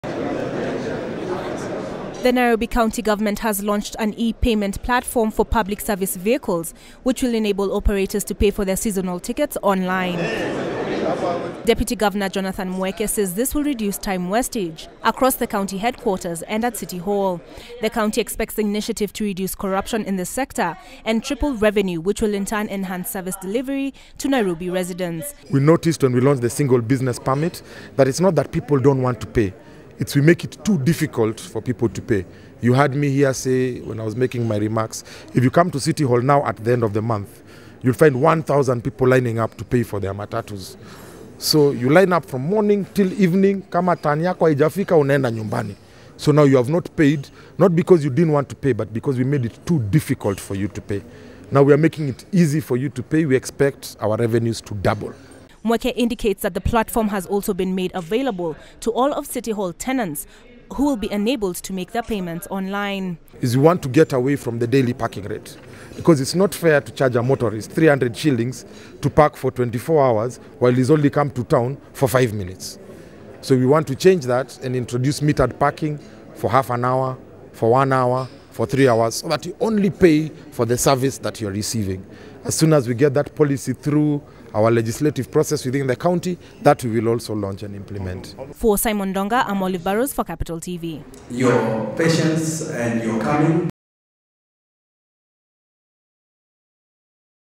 City Hall launches e-payment for PSV seasonal tickets
The Nairobi County Government has launched a platform for Public Service Vehicles (PSV) owners to pay for their seasonal tickets online.